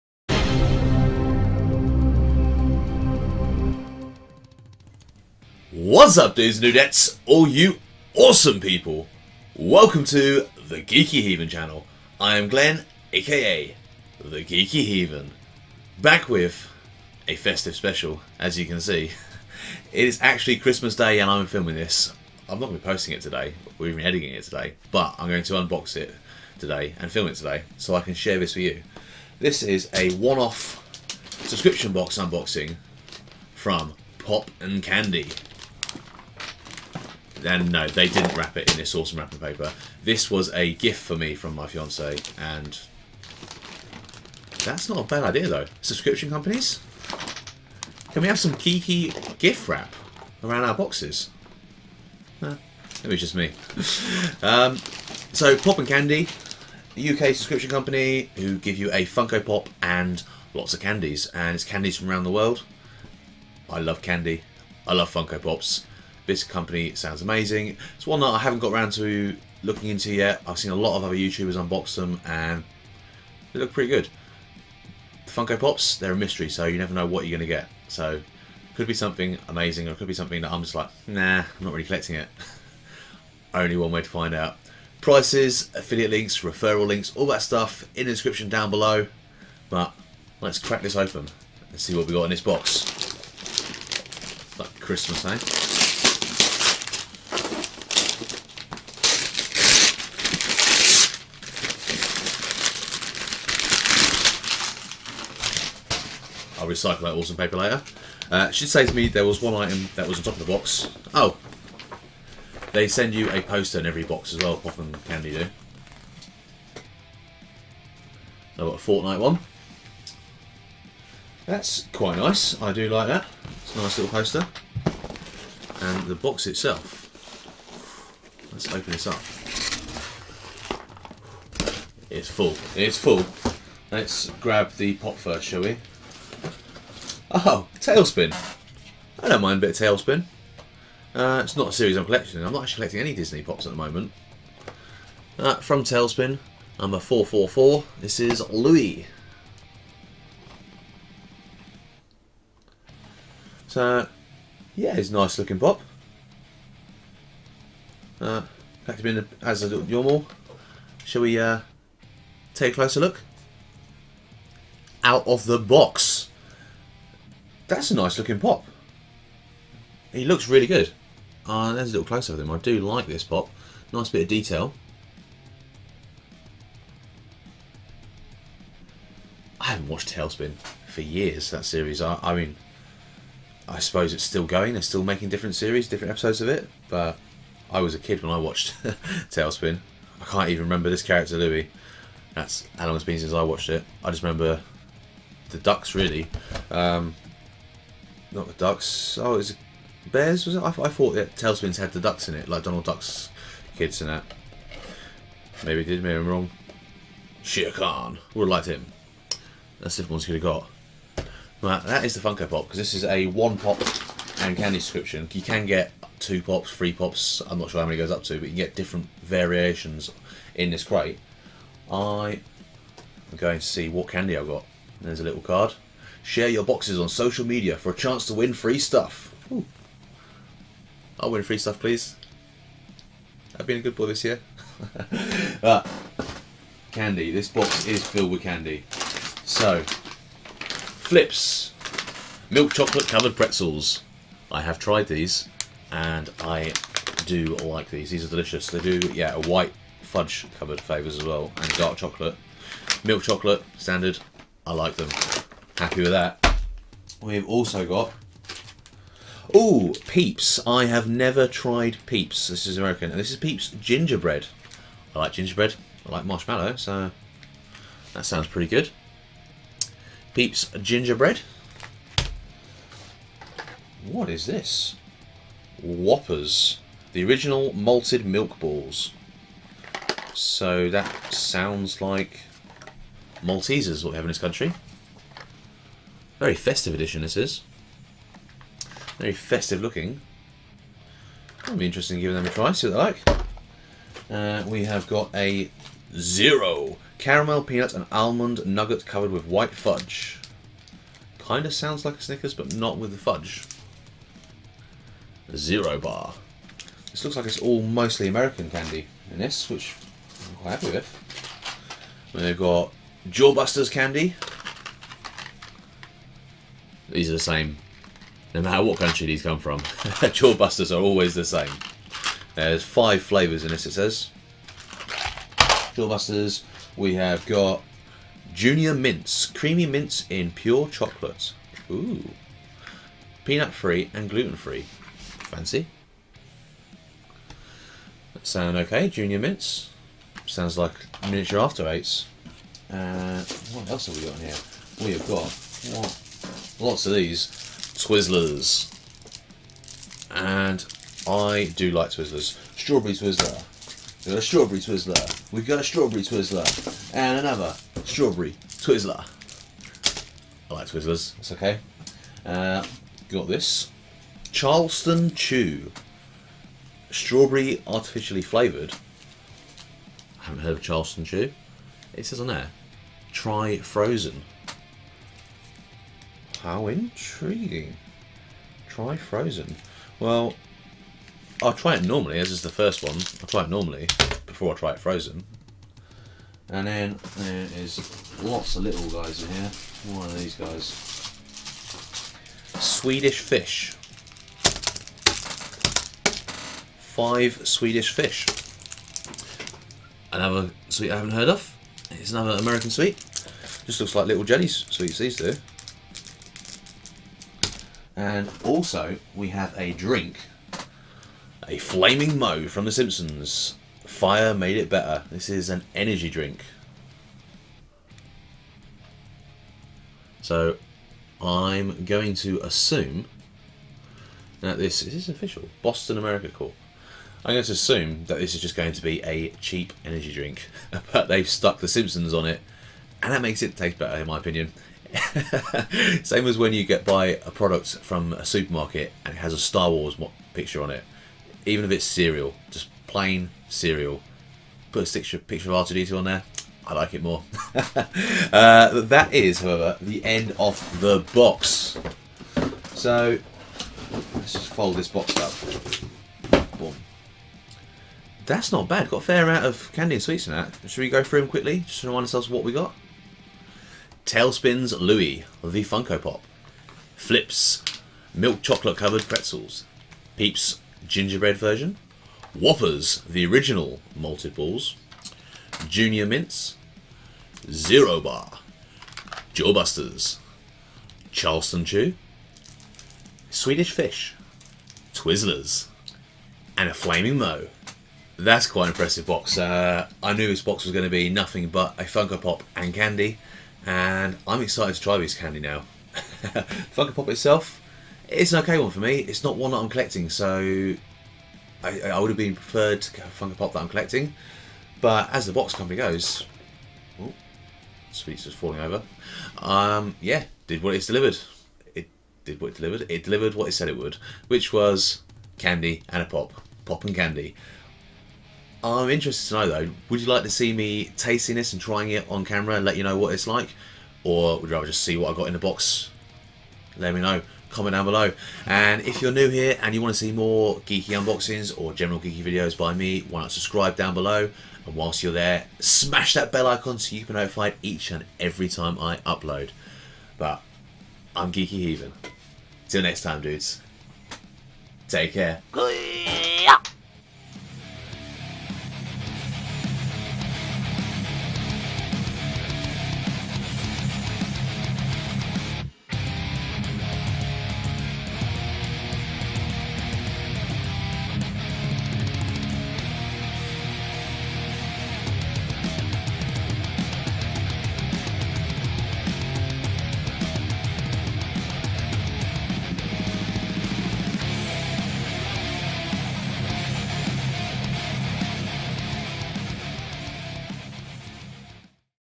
Pop N Candy December Xmas day unboxing
Haaappy Holidays,
Here is a box I have been wanting to try out for a while it is Pop N Candy.

Let me know what you think of this box and also let me know if you would like to see me taste and review the candy or just show you what comes in the box.

As always much love,

And until next time take care.

Geeky Heathen.

Infinity Crates - currently do not offer referal links but check them out @

- Funko Pop Talespin - Louie
- American Candy Hamper (similar candy to this box).